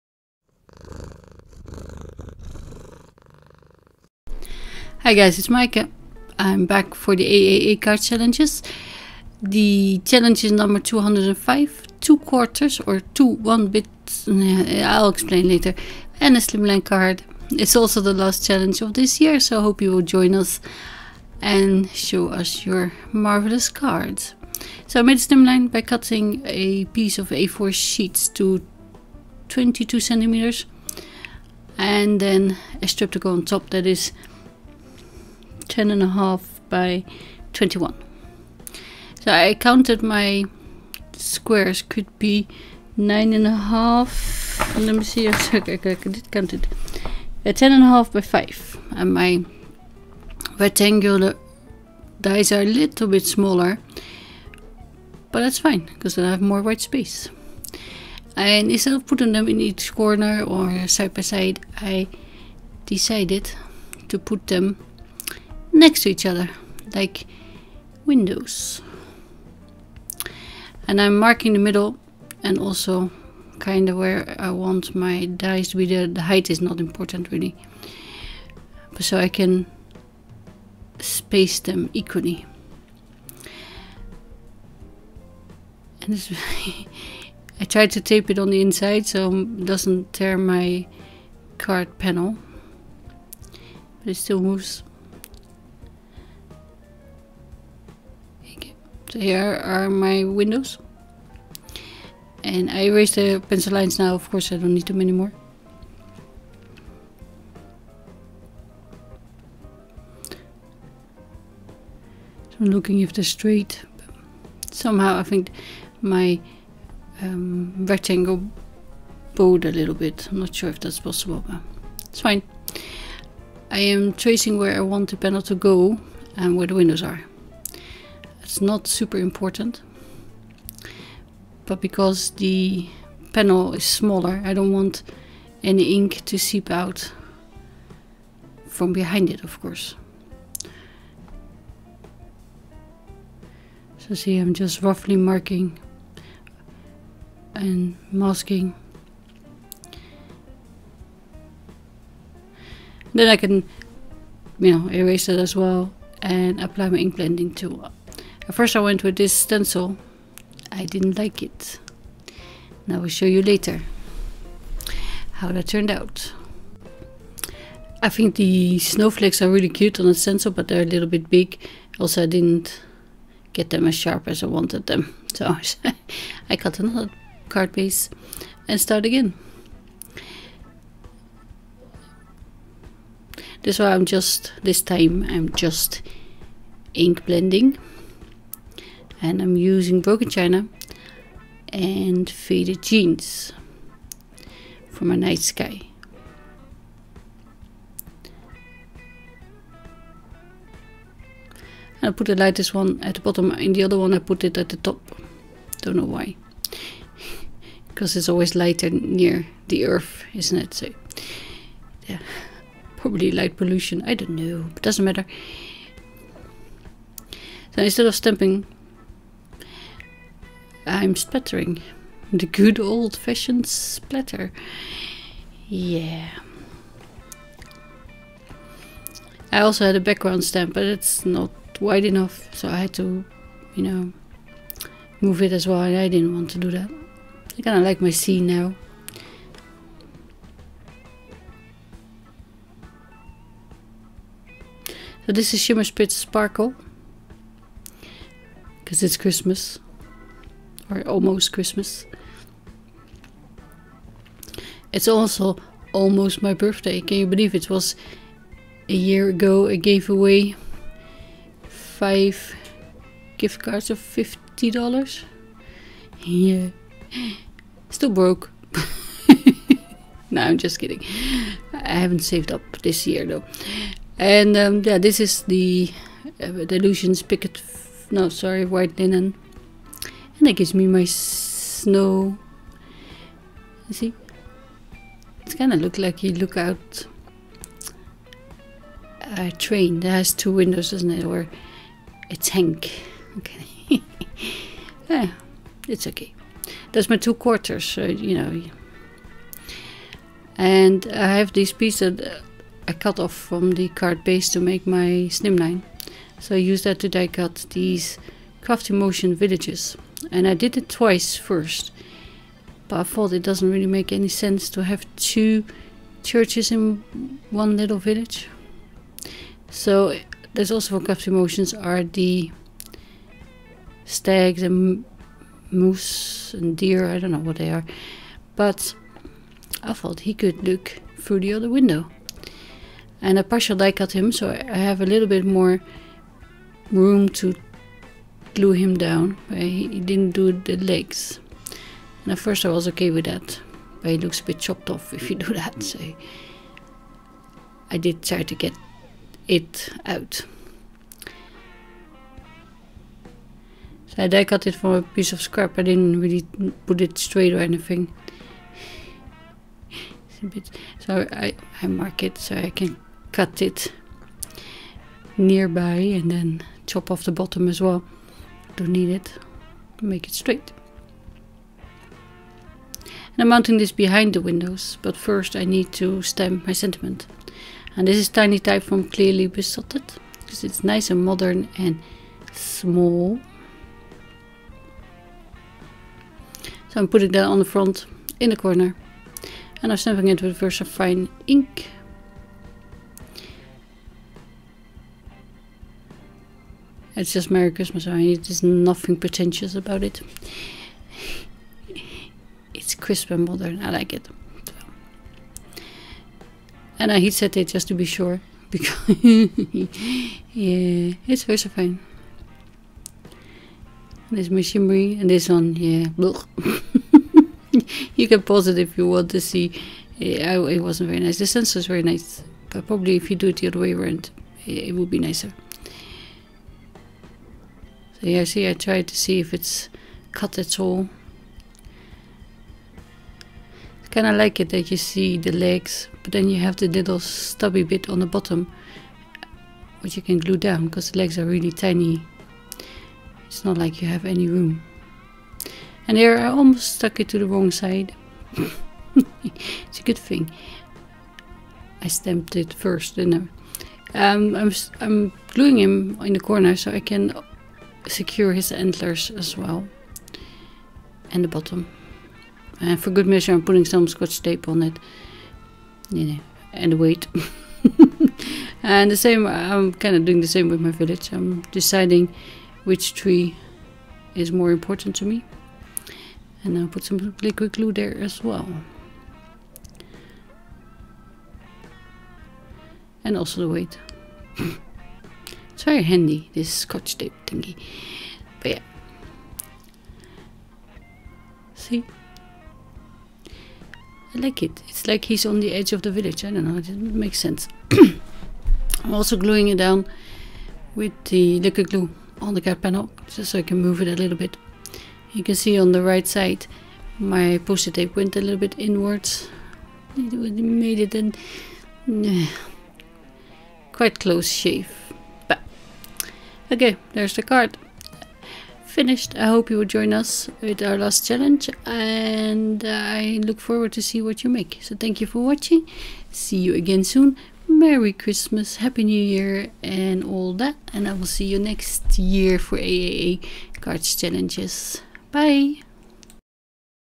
AAA Cards Challenges #205 Two Quarters + Slimline card
we've changed the "two quarters"as it was difficult for many. so now  the challenge is : Christmas with the option of a slimline.